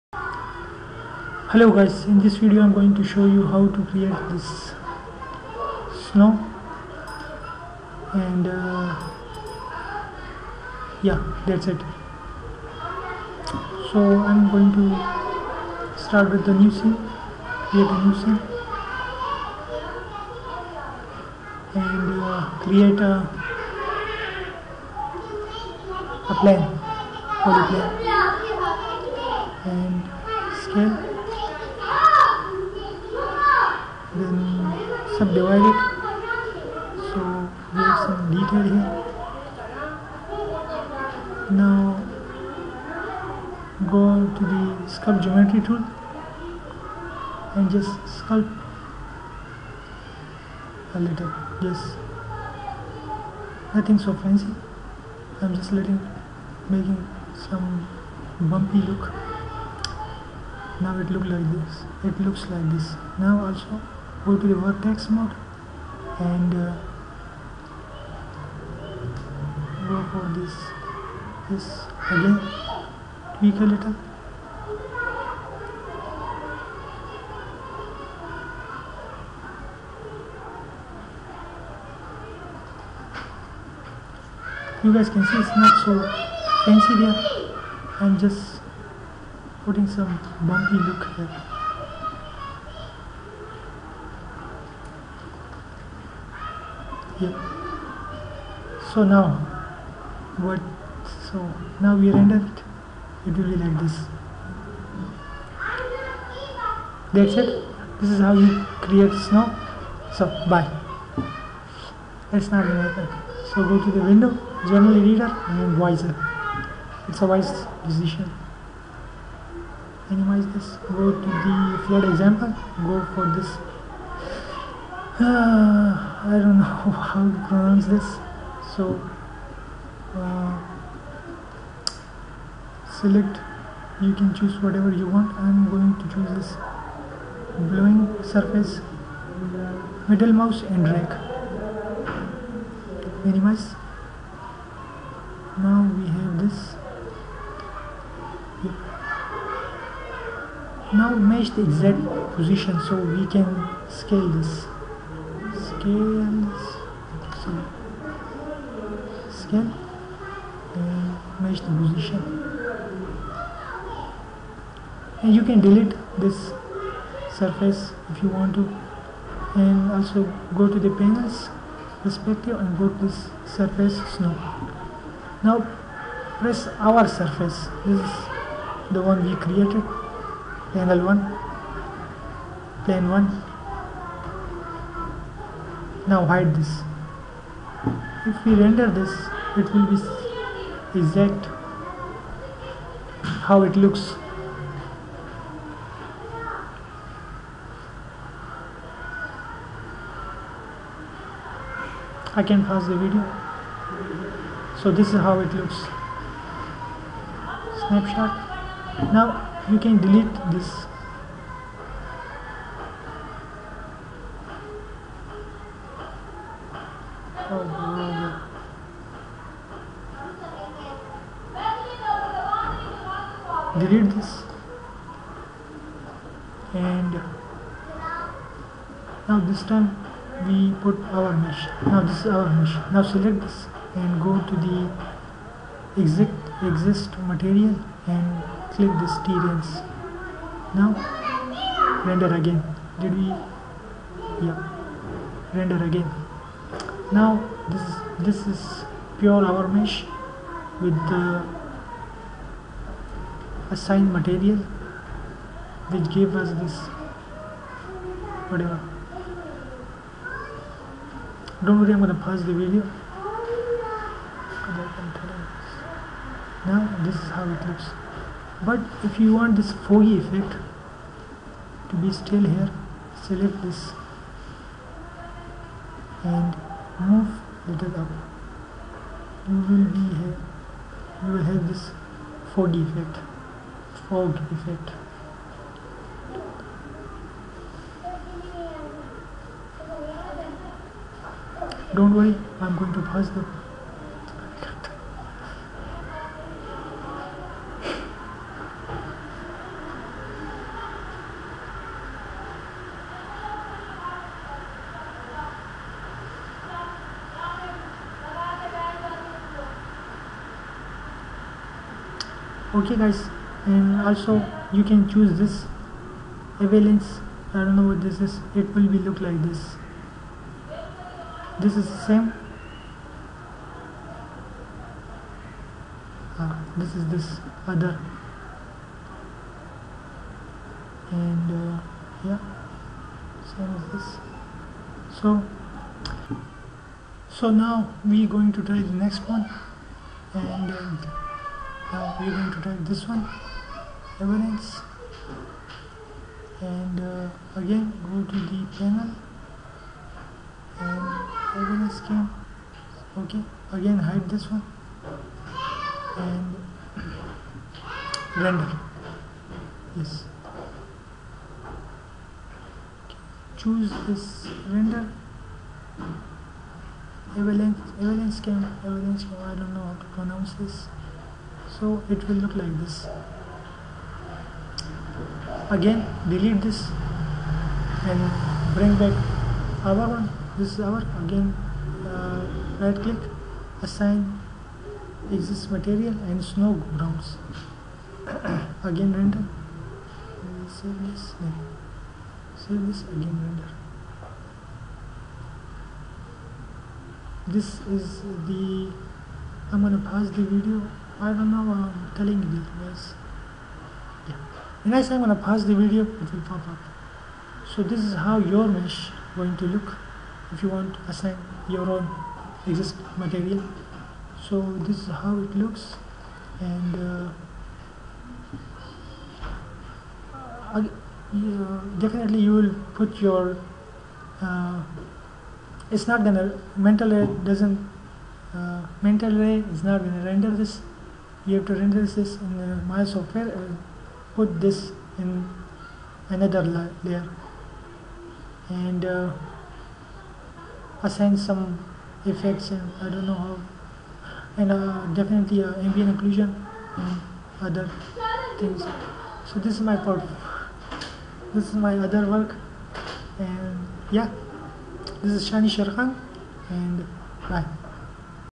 Create Snow Ground In Maya (visor)
How to create snow ground in maya and very easy and fast.
sorry about my weird accent , aand background noises , aand creating these tutorials :)